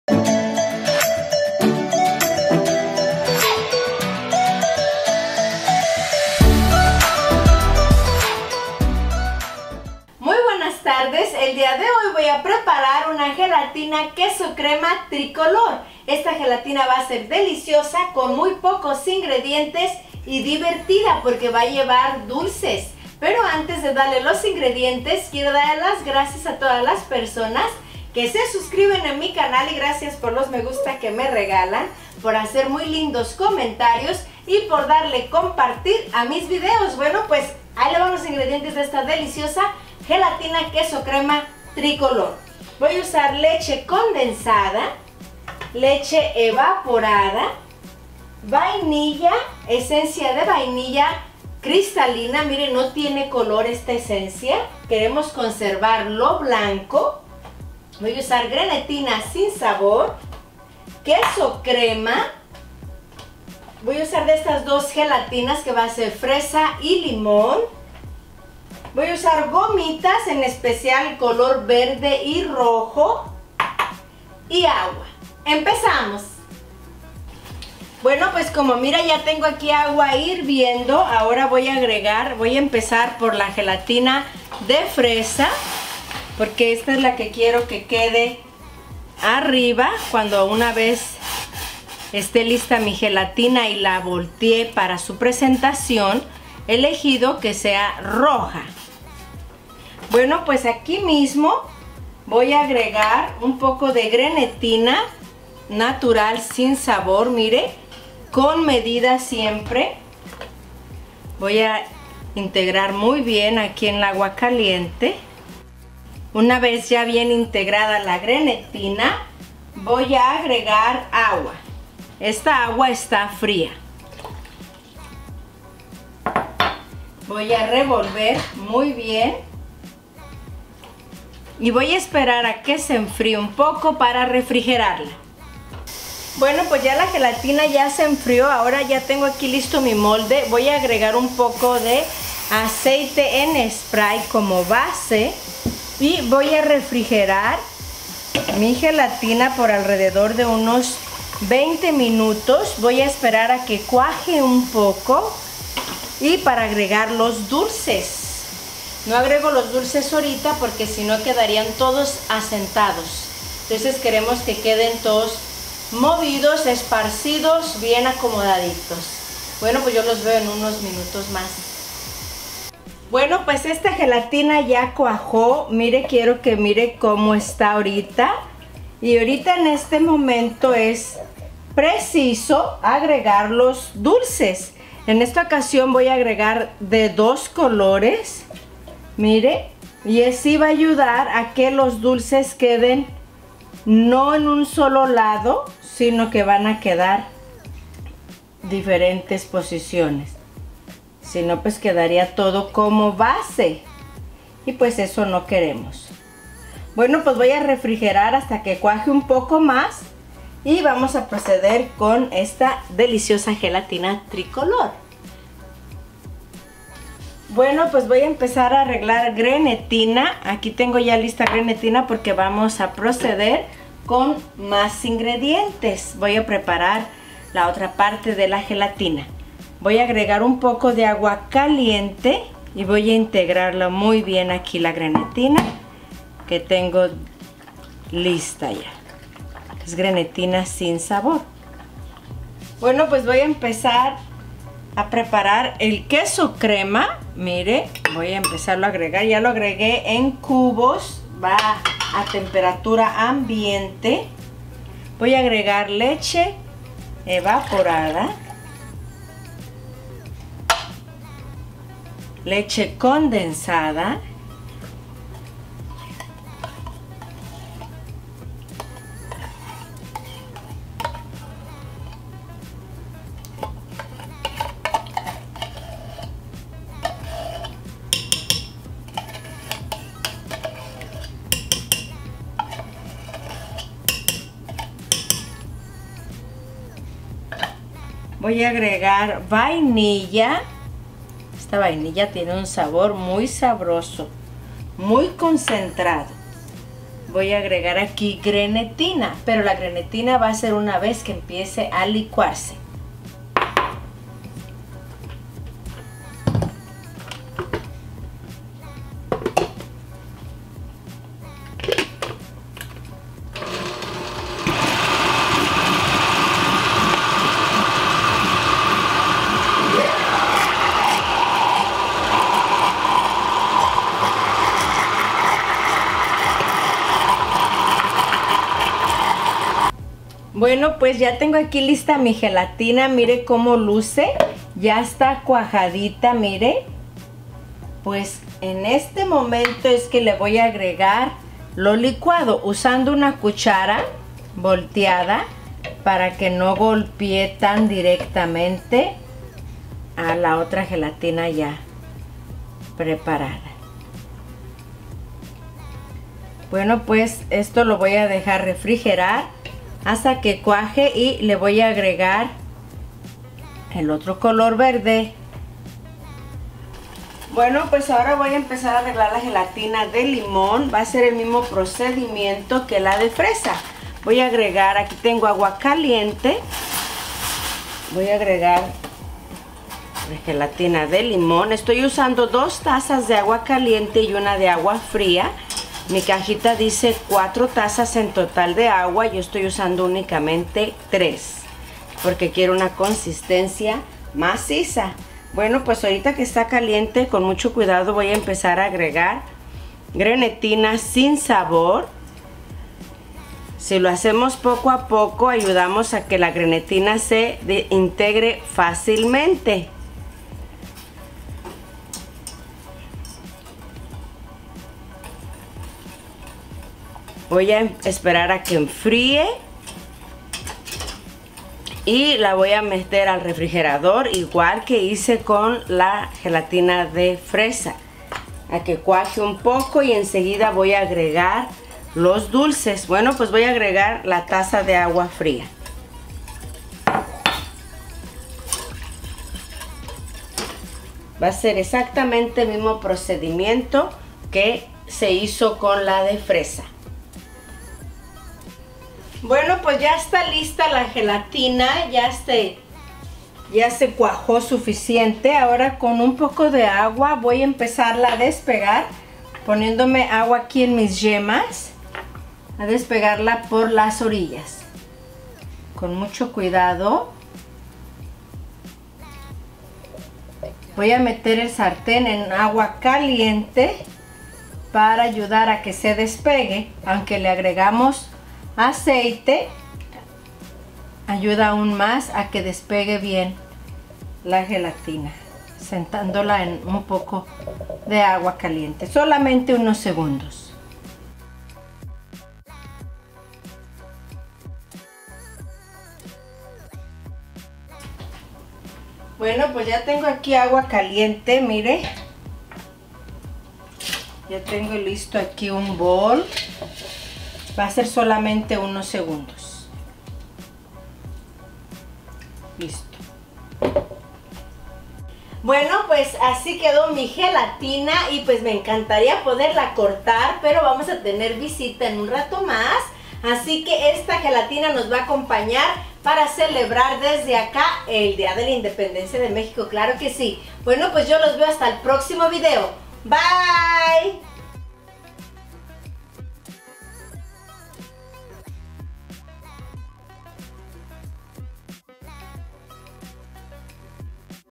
Gelatina con Queso Crema Tricolor
1.lata de leche condensada
1.lata de leche evaporada
226.grms. queso crema
1/2.cdta. de vainilla sin color
3.sobresitos de grenetina sin savor
176.grms.de gelatina savor fresa
176.grms. de gelatina savor limon
dulces gomitas la necesarias
2.cdta. de grenetina sin savor